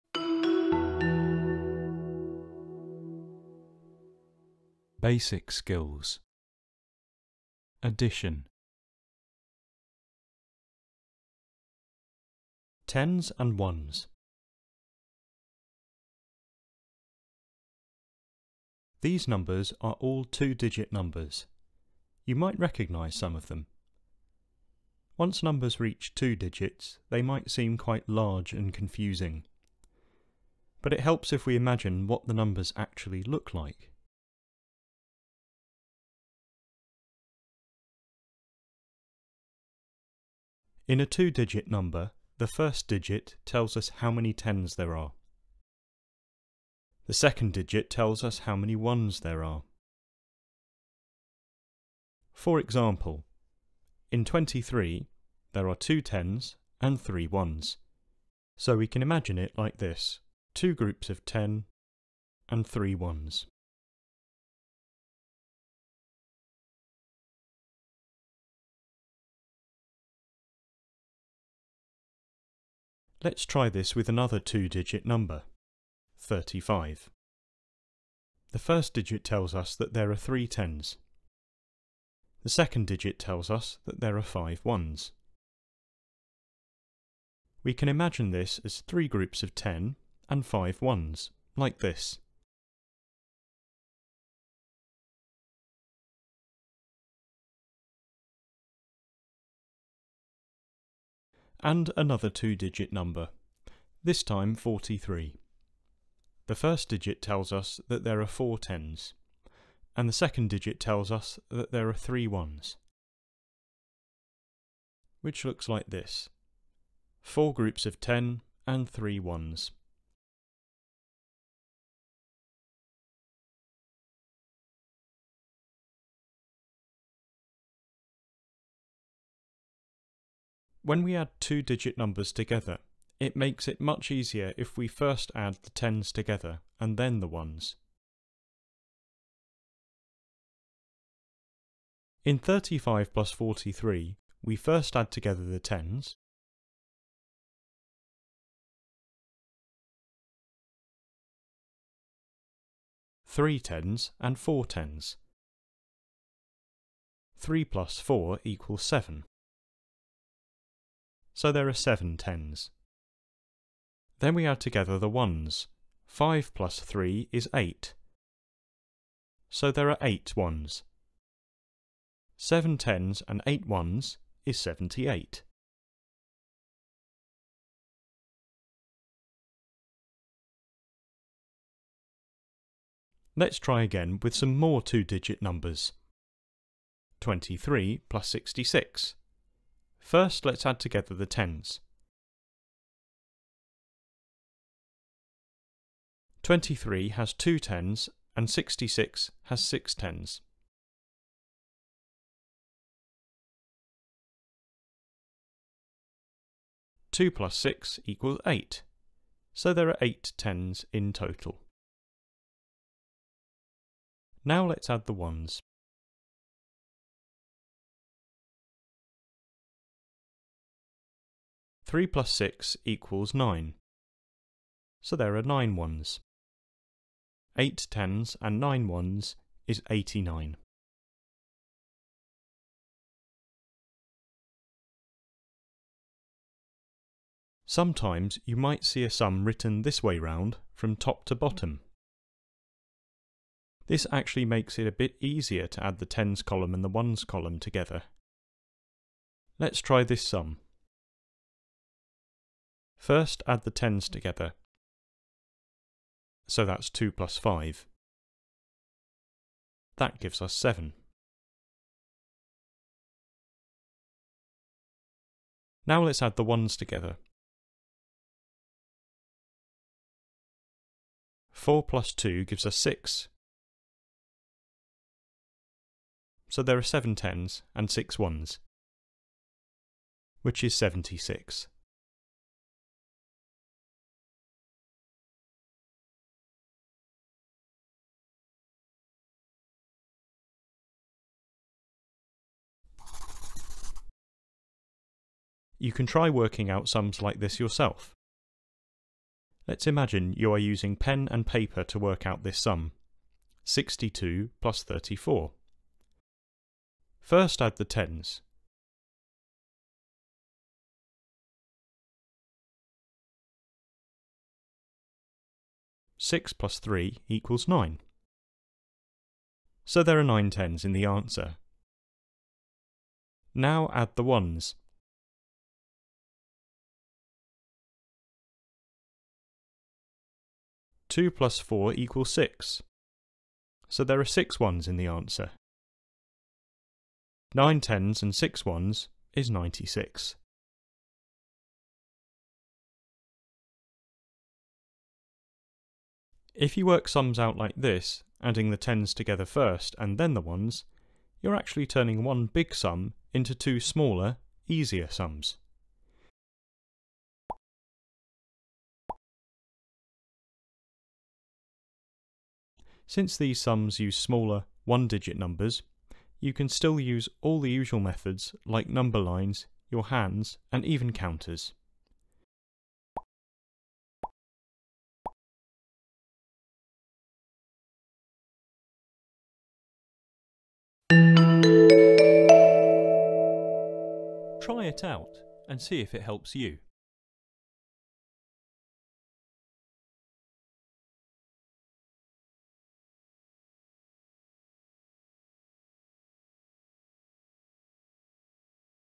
Addition: tens and ones
Shem explains how to identify and add up two-digit numbers by sorting them into tens and ones.